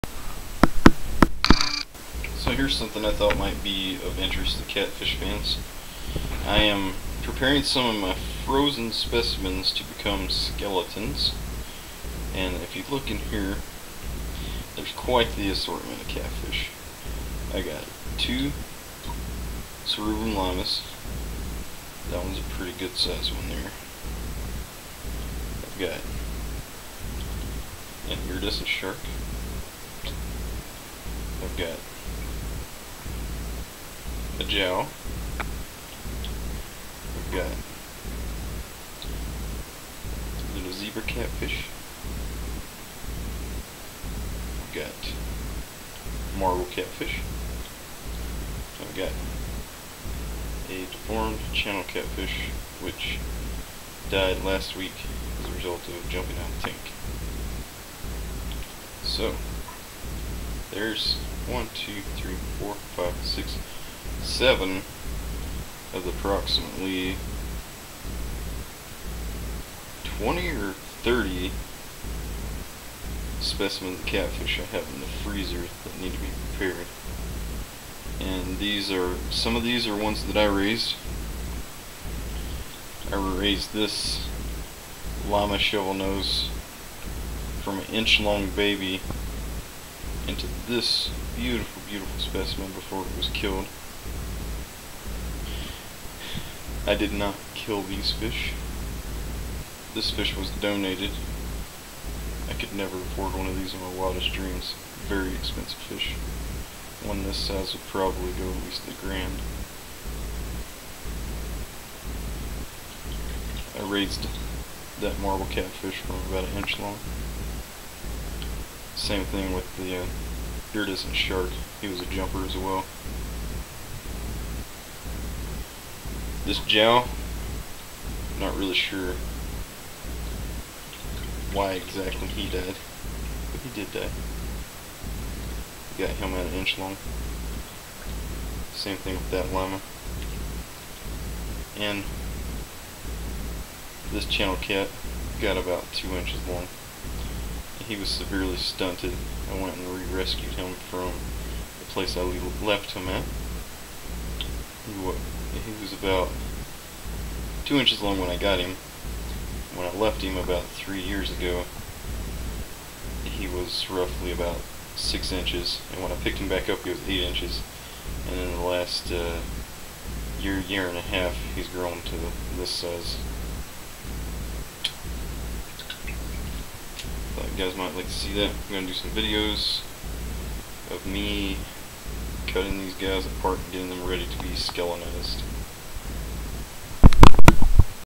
Various catfish species waiting dissection.
These are various species of catfish that I am preparing the skeletons of.  For now they will be preserved in alcohol until I am living where I can get a colony of dermestid beetles going again.